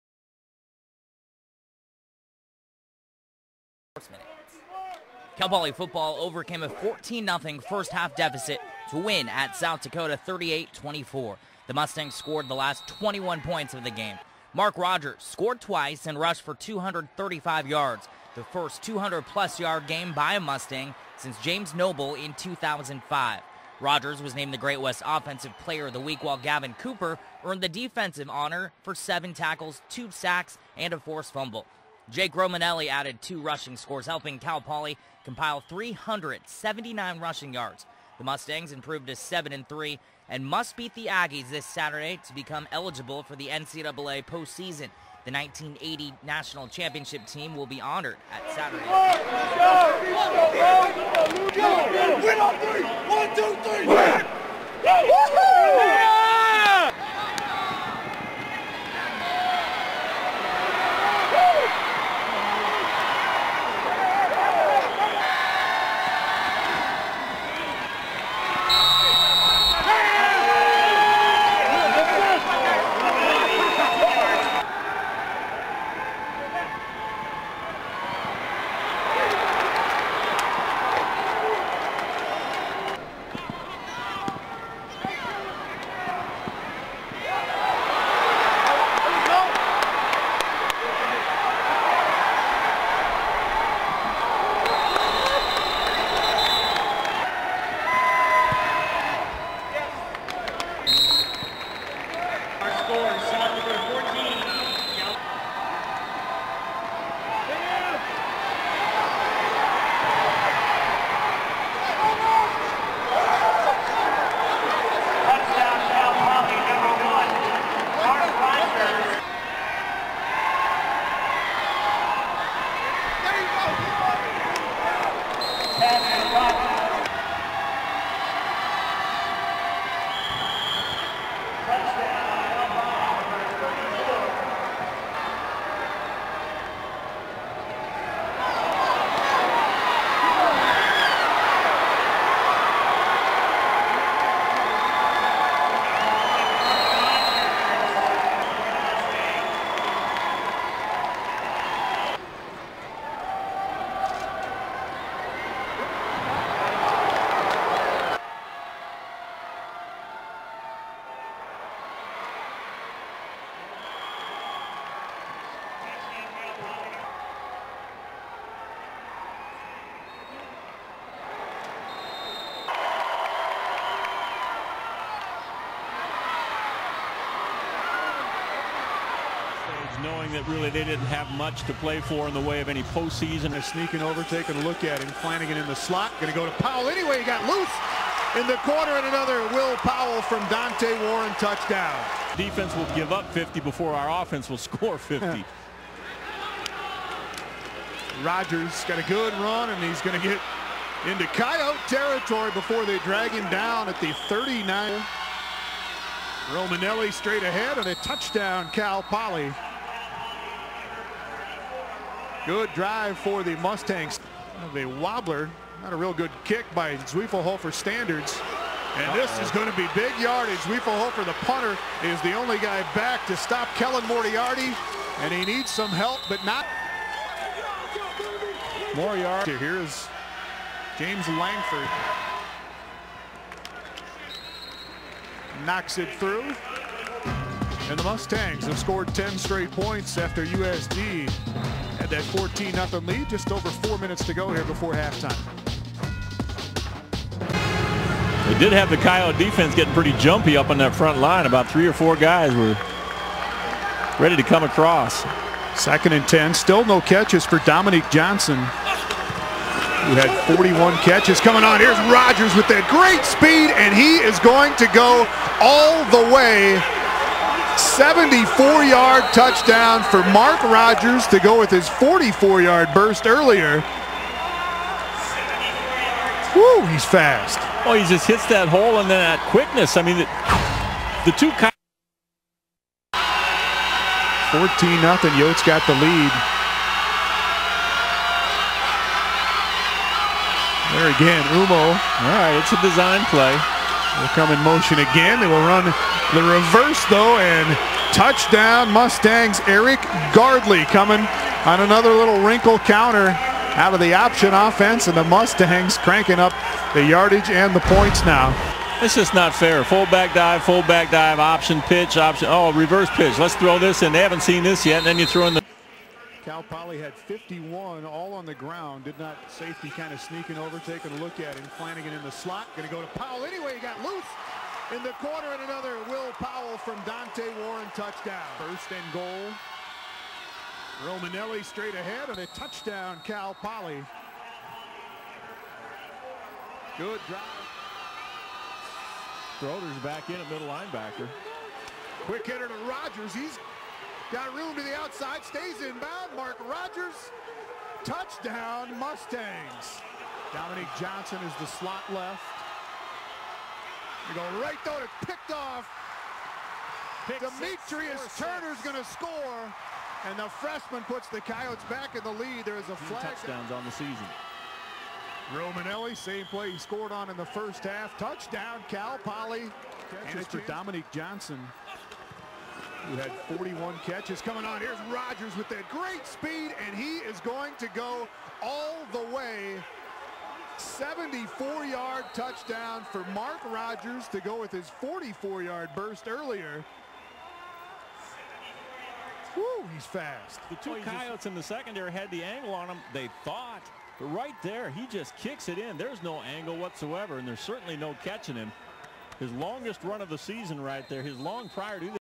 Cal Poly-South Dakota Football Highlights
Highlights of Cal Poly's 38-24 victory over South Dakota on Saturday, Nov. 6, 2010, in the DakotaDome at Vermillion, S.D. Video courtesy of KSBY and KCOY.